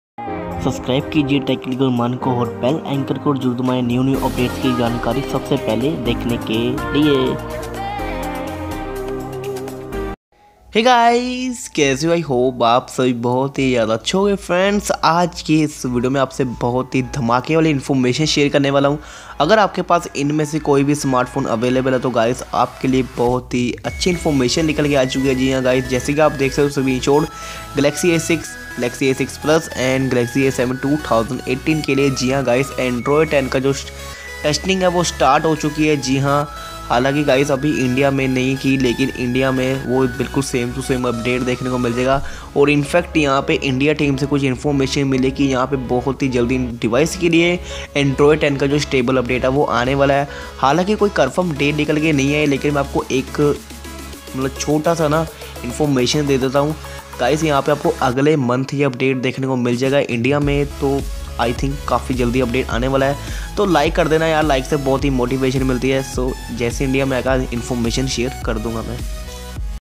Samsung phones good news Android 10 testing start one UI ke sath
Hi guys aaj ki is video mein aapko bataunga Samsung Galaxy A7 2018 Galaxy A6 Plus Galaxy A6 Android 10 update starting testing full explain Hindi

New new software update news information share daily tech video upload

Ummid aapko video Pasand Aaegi💜💜💜💜